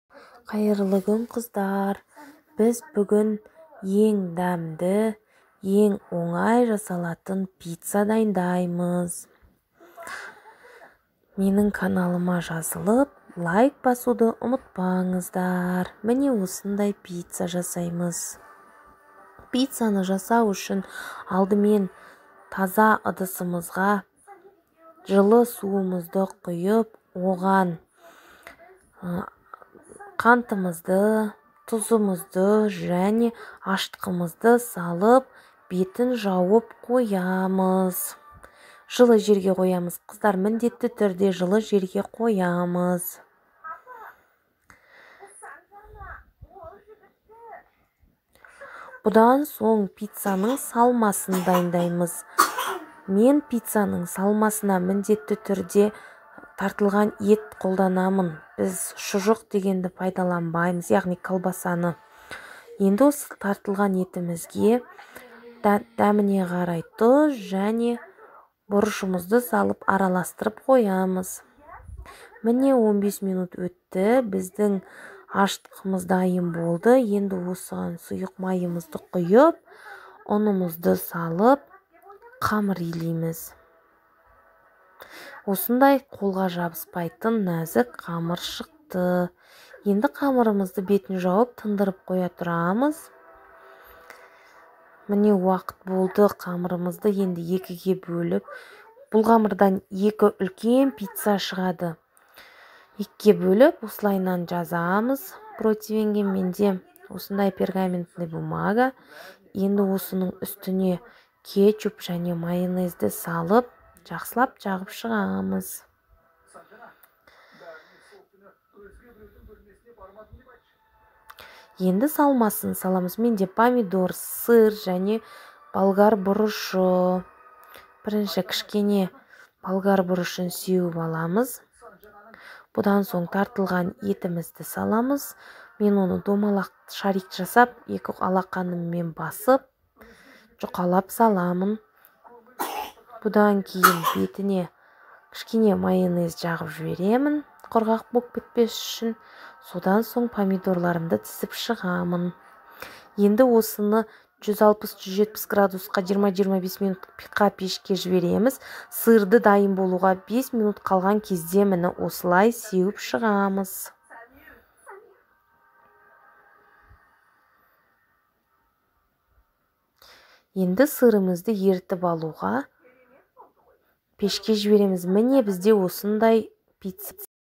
Ең дәмді әрі ең оңай дайындалатын ПИЦЦА. Қазақша рецепт.
Құрамы:
Қамыр үшін:
• жылы су 300 мл;
• ашытқы 2 шай қасық;
• қант 2 ас қасық;
• тұз 1 шай қасық;
• сұйық май 120 мл;
• ұн 700-800 гр.

Салмасына: ( өз қалауларыңызша)
• тартылған ет 300-400 гр;
• татымына қарай тұз,бұрыш;
• болгар бұрышы 2-3 дана;
• қызанақ 2-3 дана;
• үккіштен өткізілген ірімшік(сыр);
• кетчуп немесе томат пастасы ;
• майонез немесе қаймақ.

Жасап көріңіздер, Ас болсын! 😘

Сіздермен болған мен, Әсел.